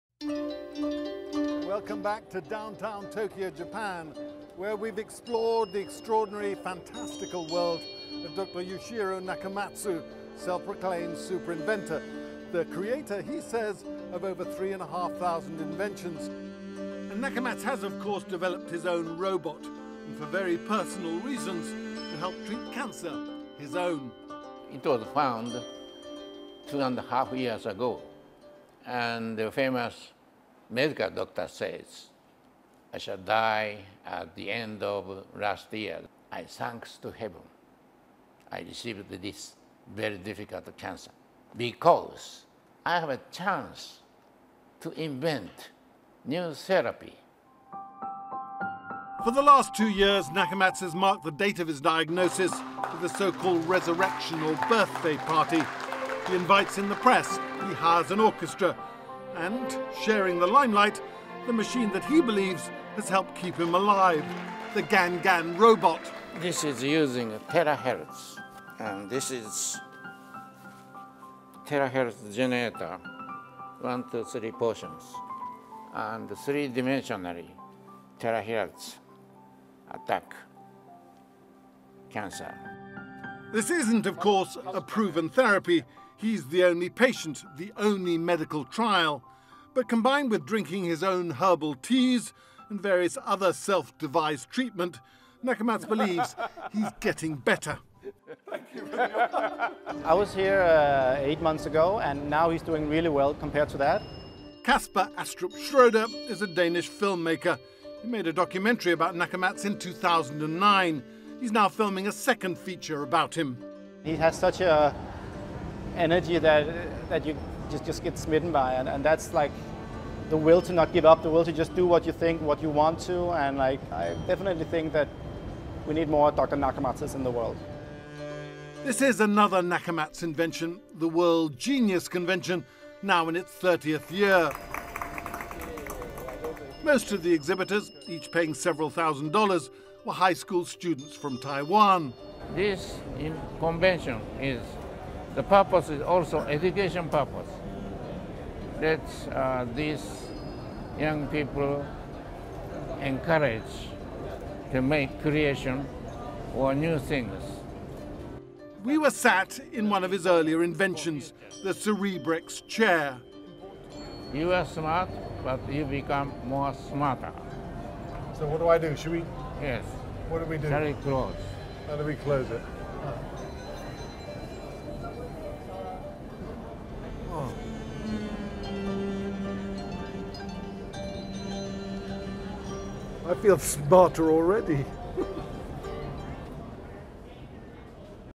This man is the guinea pig for cancer therapy he invented
Serial inventor Yoshiro Nakamatsu is testing out a new cancer-fighting therapy of his own design.